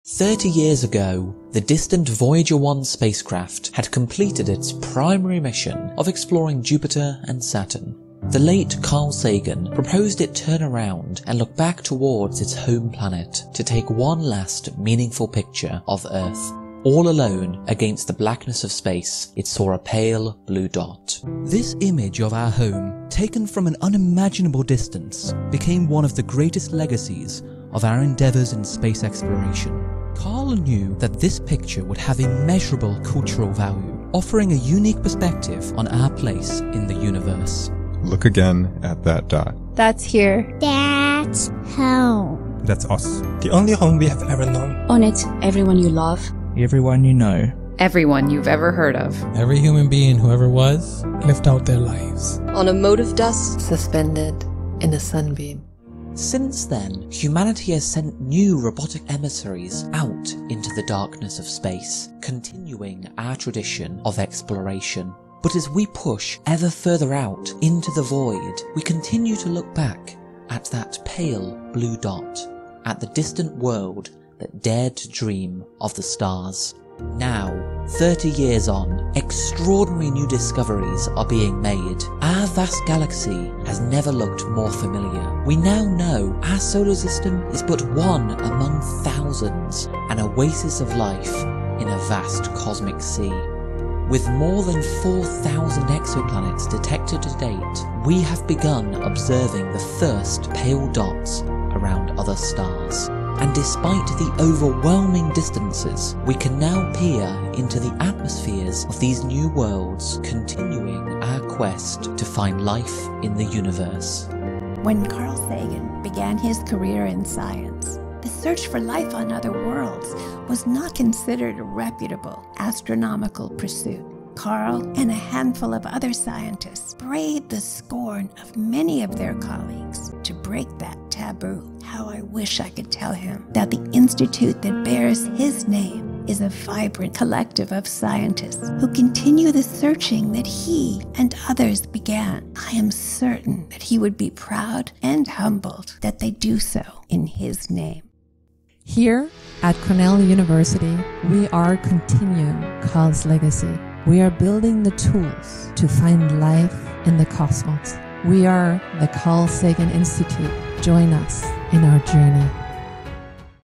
Carl Sagan's Pale Blue Dot - 30 Years On
30 years ago, Carl Sagan requested the Voyager 1 spacecraft take one last picture of Earth. This is the legacy of the Pale Blue Dot. Join us in our search for life in the Universe.

Pale Blue Dot text courtesy of Ann Druyan.

Special thanks to Ryan MacDonald, Jack Madden, Thea Kozakis, Lara Sky Kaltenegger, Mark Sarvary, Zifan Lin, Maryame El Moutamid, Andrew Ridden-Harper, Nikole Lewis, Jake Turner, Ishan Mishra, Zoe Ponterio, Lisa Kaltenegger, and Ann Druyan for lending their voices.

The Carl Sagan Institute logo was created by Christo Downs.

Further information and links